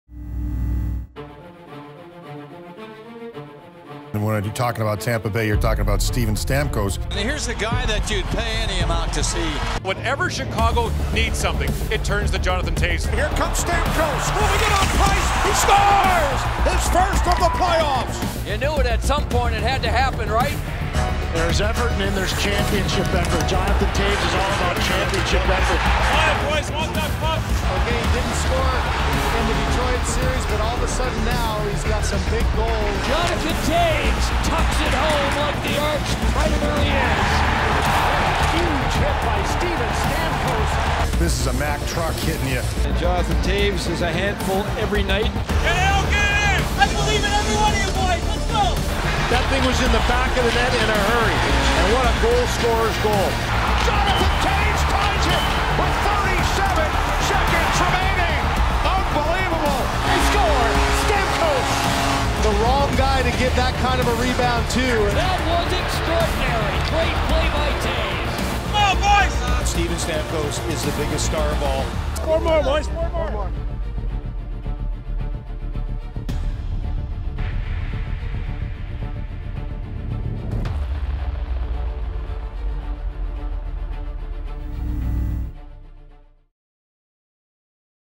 Star Spotlight: Jonathan Toews vs Steven Stamkos
They lead by their teams and now go head to head for hockey's prized possession.